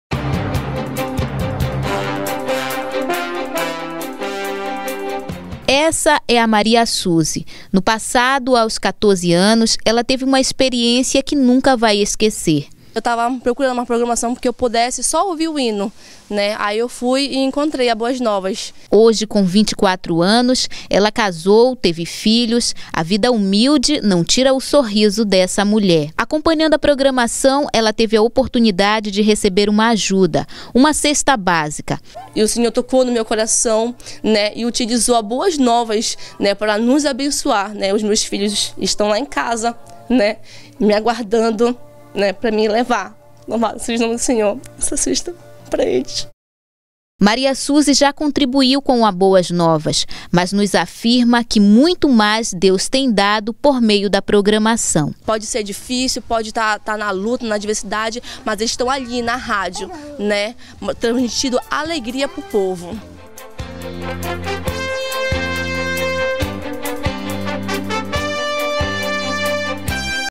Testemunho Boas Novas - Maria Suzy
Semeadora da Boas Novas visita a emissora. (INFORME AD 364)